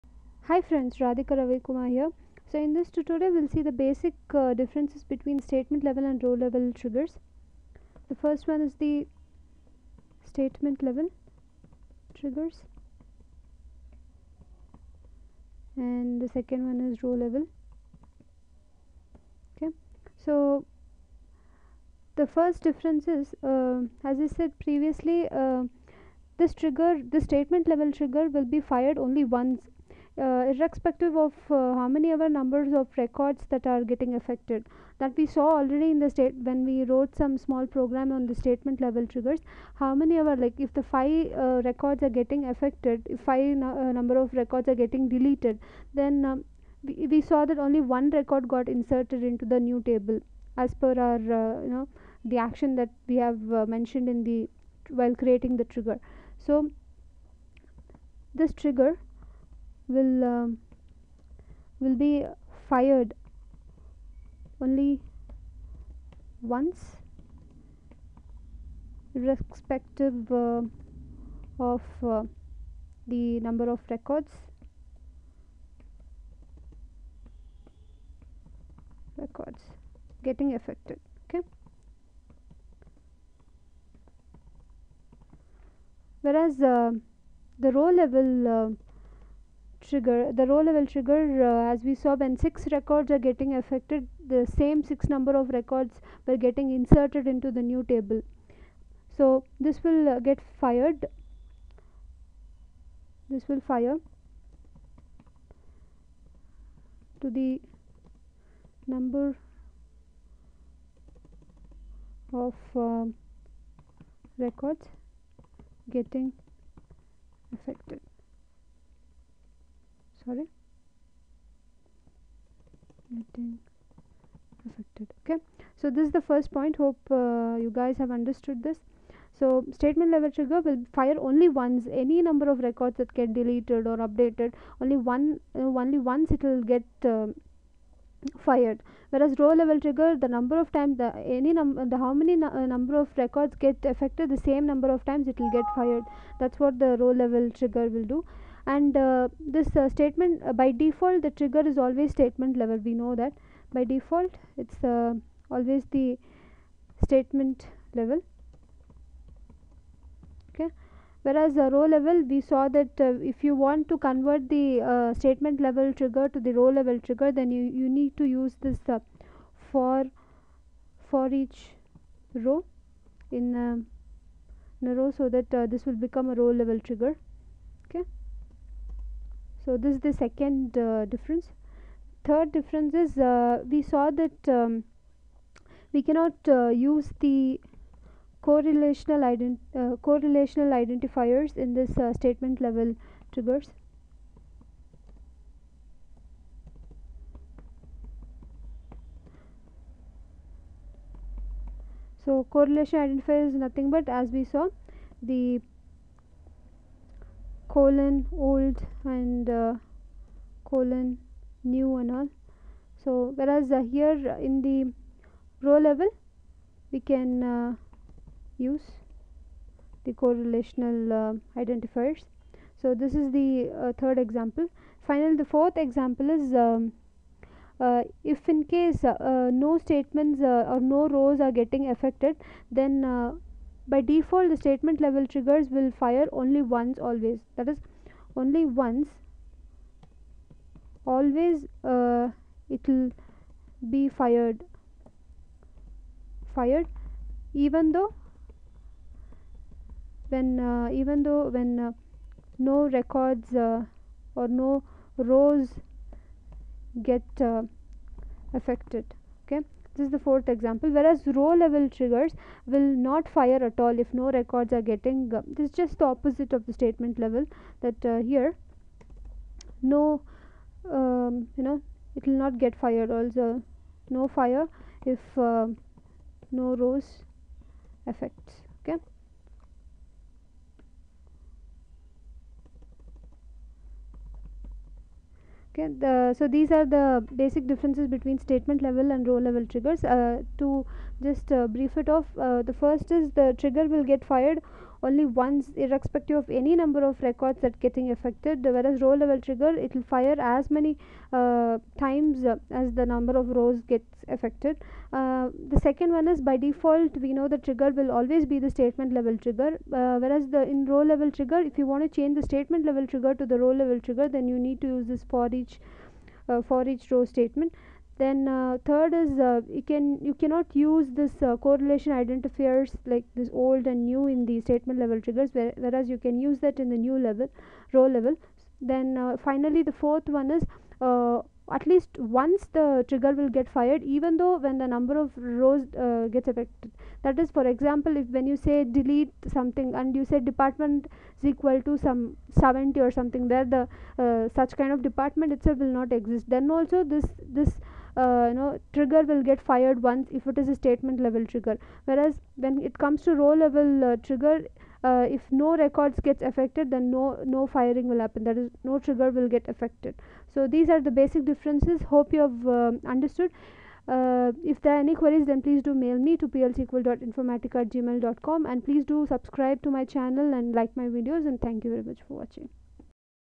PL/SQL: Triggers-Difference between Statement & Row level
In this tutorial , you'll know the difference between statement and row level triggers..